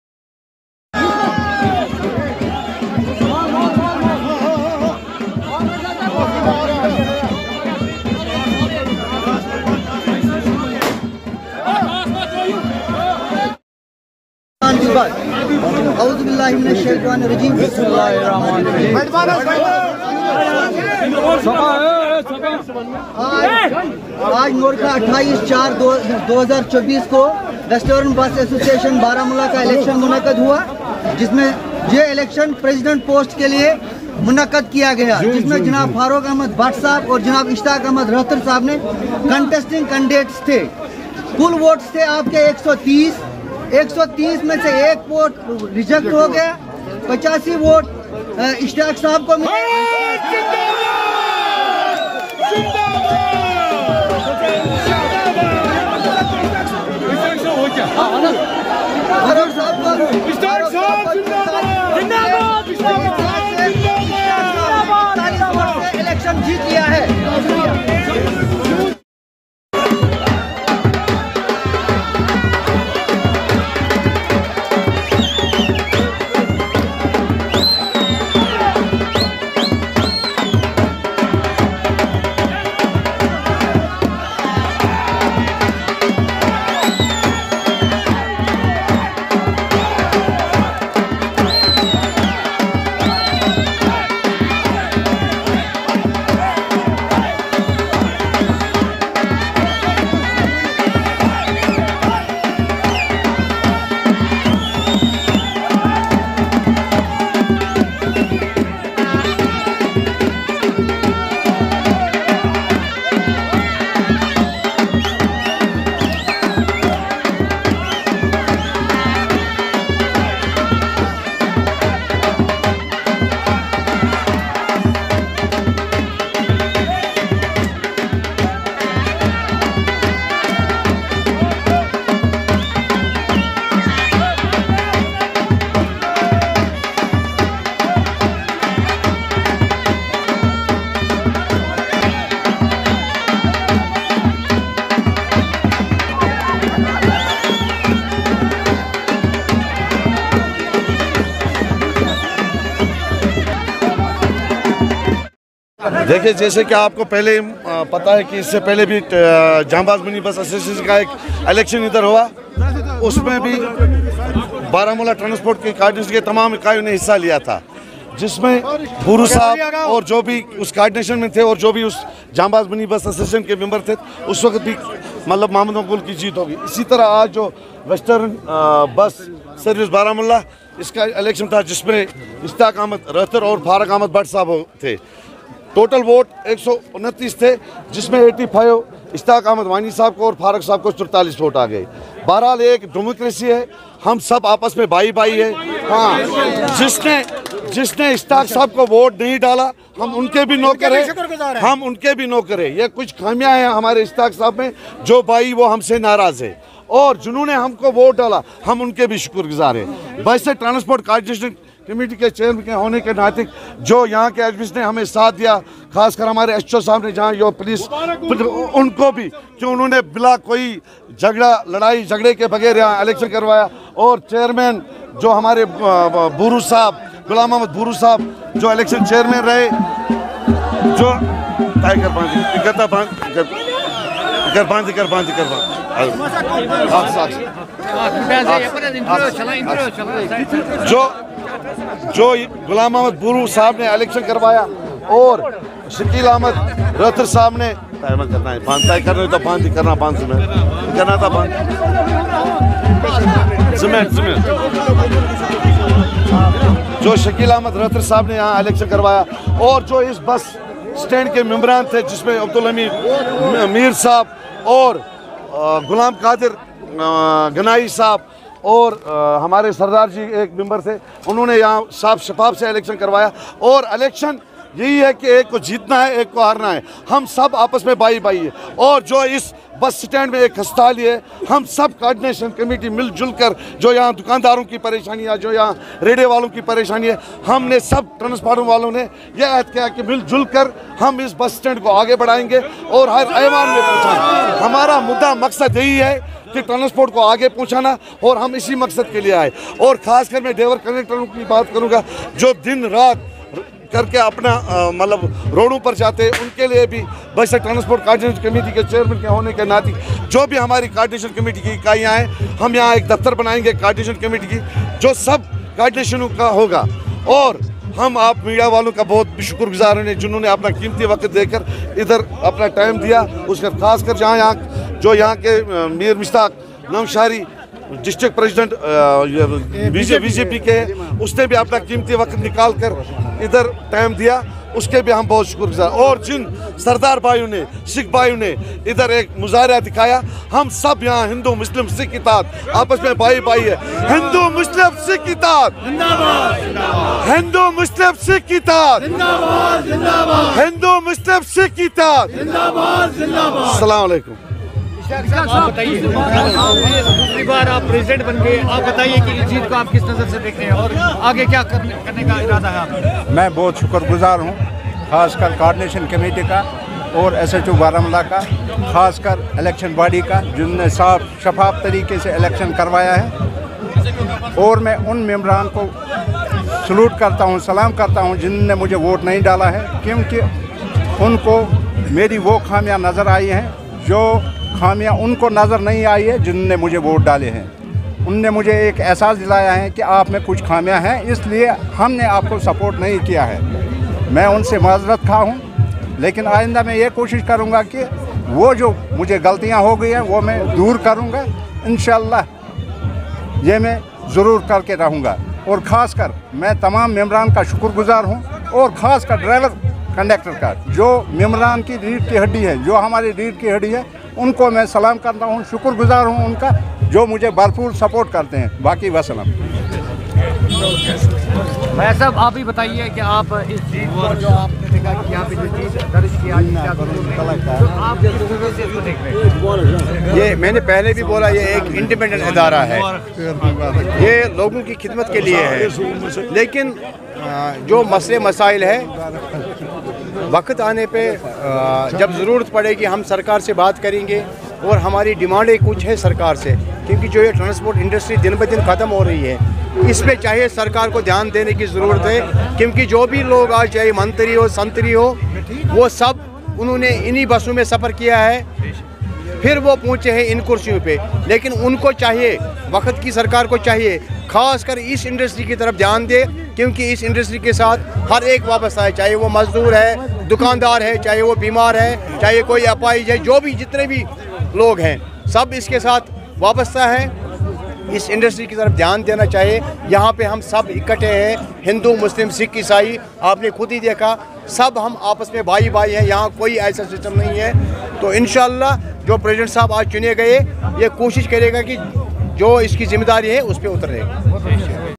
Western Bus Stand Baramulla Election Ishtiaq Ahmed Rather has won the electon Total number of 130
Western Bus Stand Baramulla Election Ishtiaq Ahmed  Rather has won the electon  Total number of 130 votes were recorded, of which Ishtiaq Ahmed got 85 votes and Farooq Ahmed  bhat got 44 votes, thus Ishtiaq Ahmed won the Westeren Bus stand    Election. Board Chairman Ghulam Deen Buhro was the chief election officer in his supervision election was conducted smoothy.